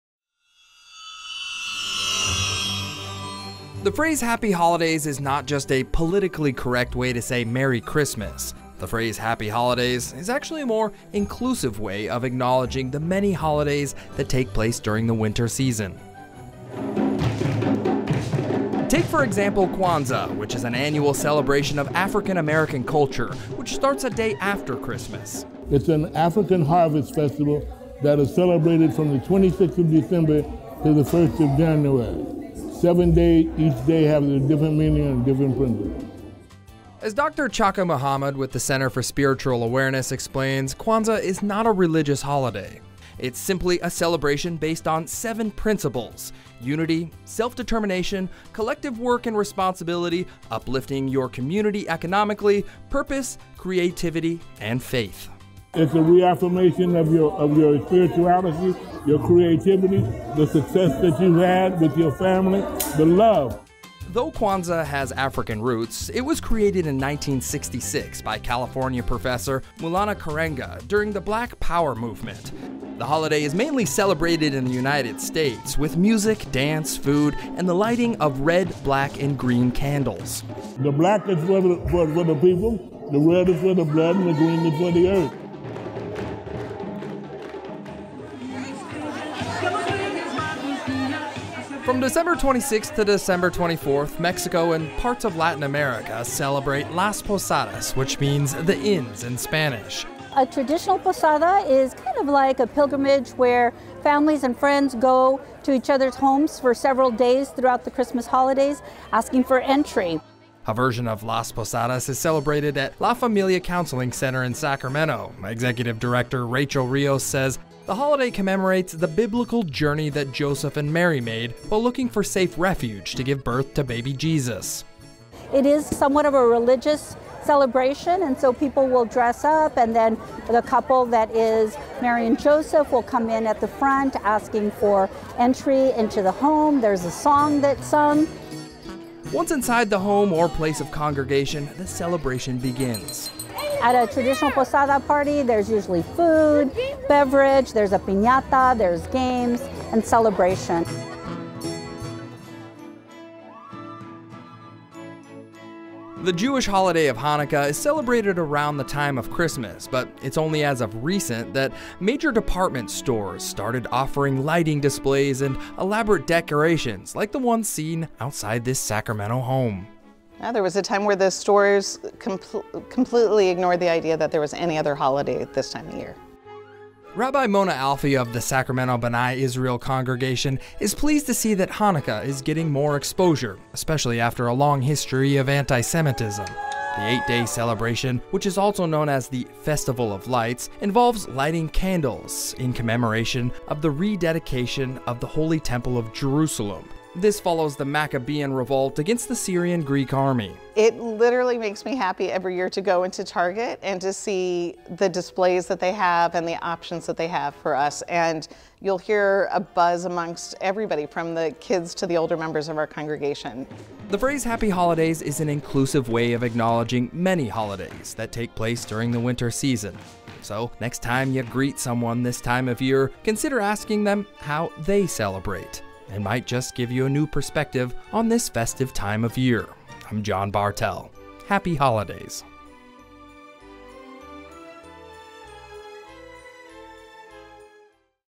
An inclusive look at the holiday season | Race and Culture
The holiday season includes recognition of a variety of winter celebrations taking place at this time of year.